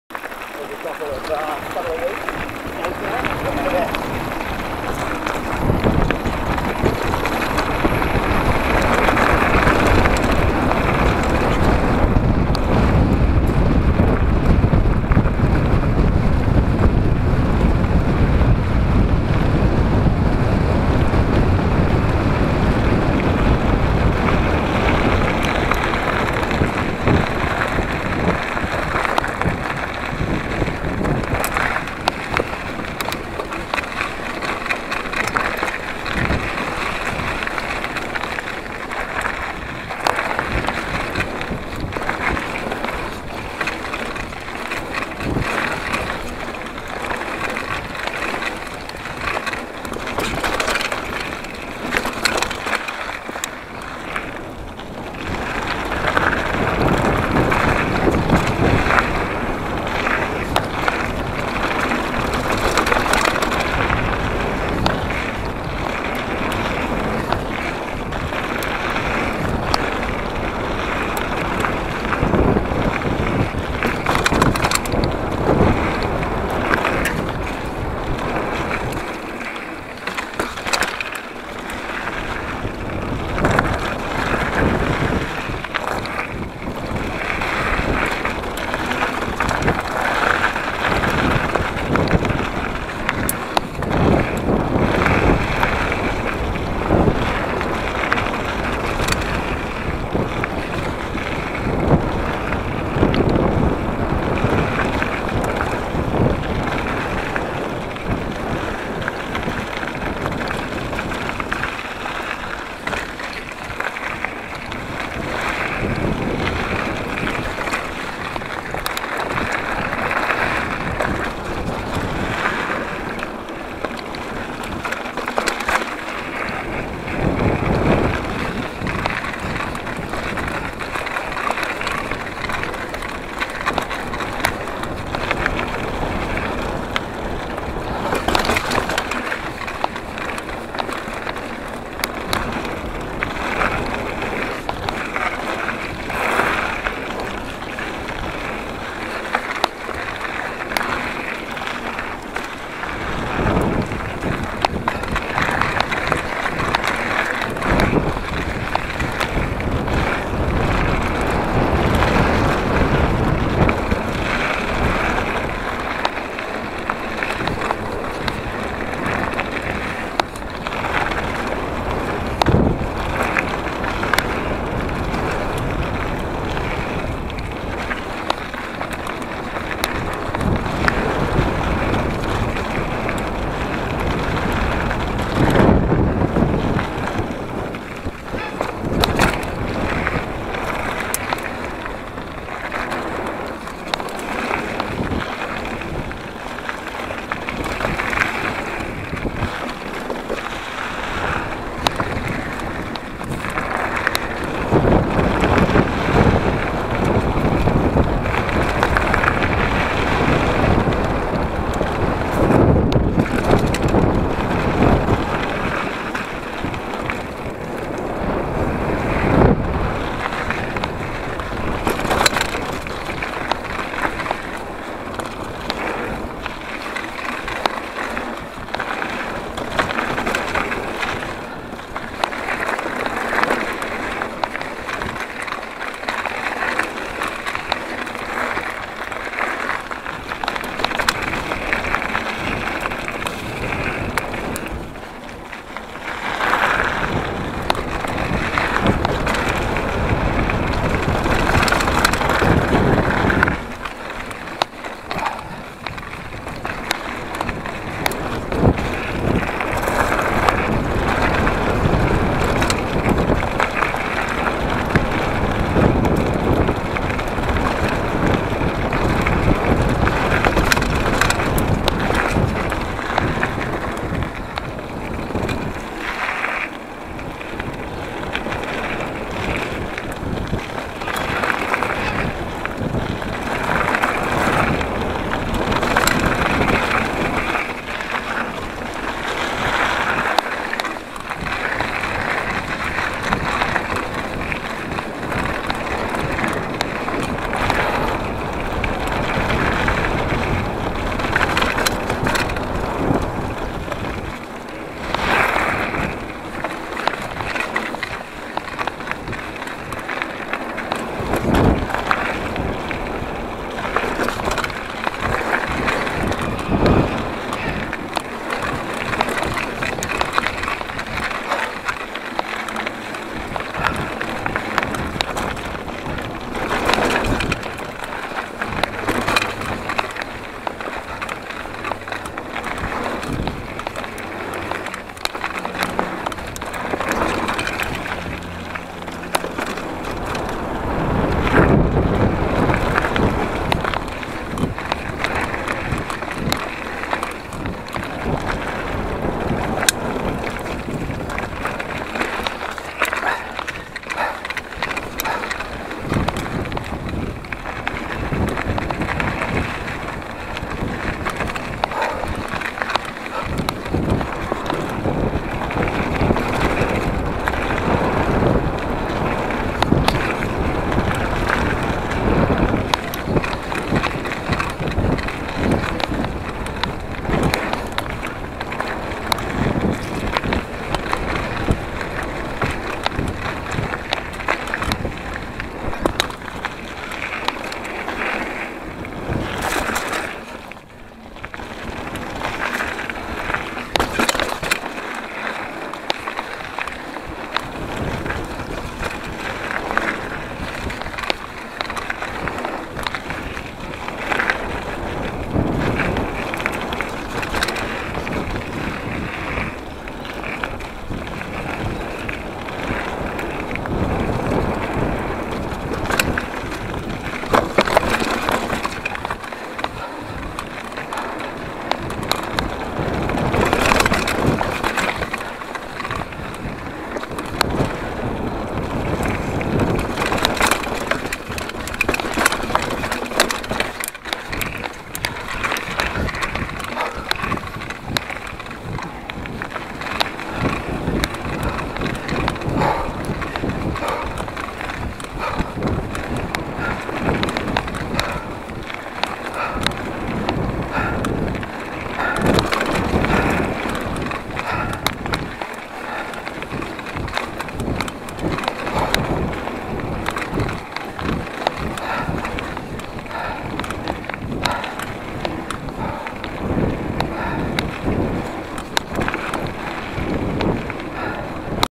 Kombi Express (MTB Trail) - Sep 2016
BIKE: 2014 Giant Trance 3 27.5 with 40mm Renthal Apex Stem & 760mm Wiser Handlebars.

Trail is located at Samford Conservation Park, Brisbane.

FYI - I don't do ANY jumps!  After breaking 9 x ribs 8 months prior, I've decided to keep both my wheels on the ground as much as possible.  So I upgraded both my tyres to High Roller II and started focusing on Traction, rather than trying to get 'Sweet Air' !  ;)